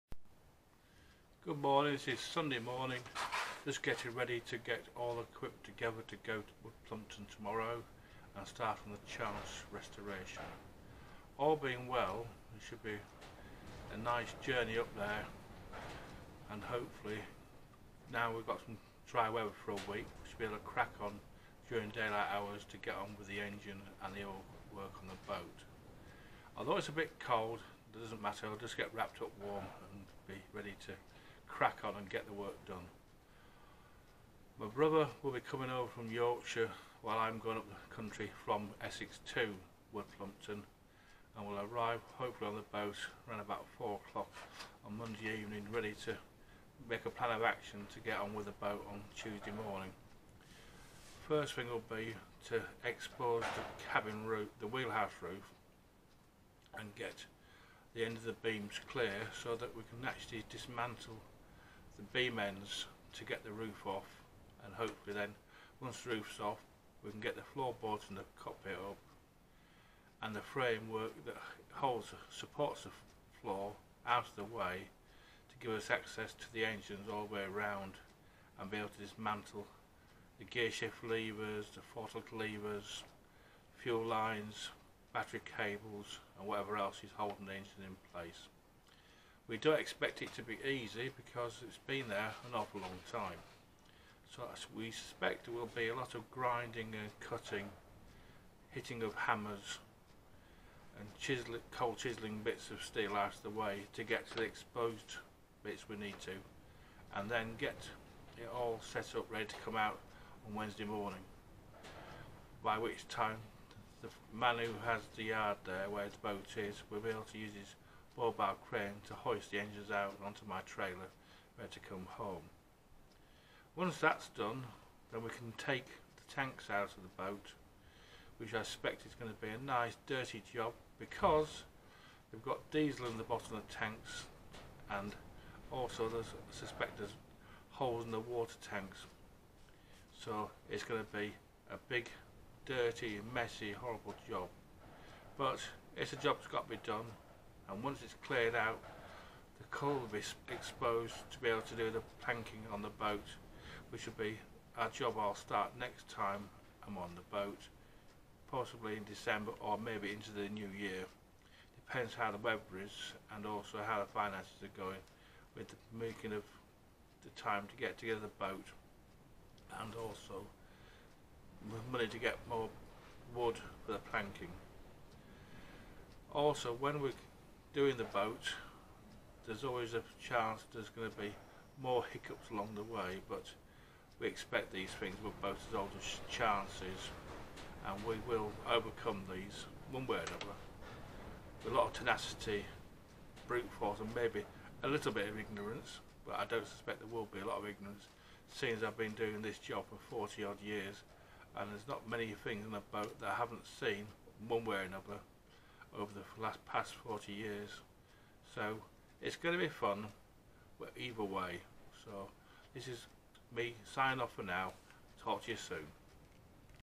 The Papendick Family's restoration of our James Silver "Western Isles" Motor Sailer "Chance"
These are the updates of their work in getting their James Silver "Western Isles" Chance
back afloat and once more sailing the high seas.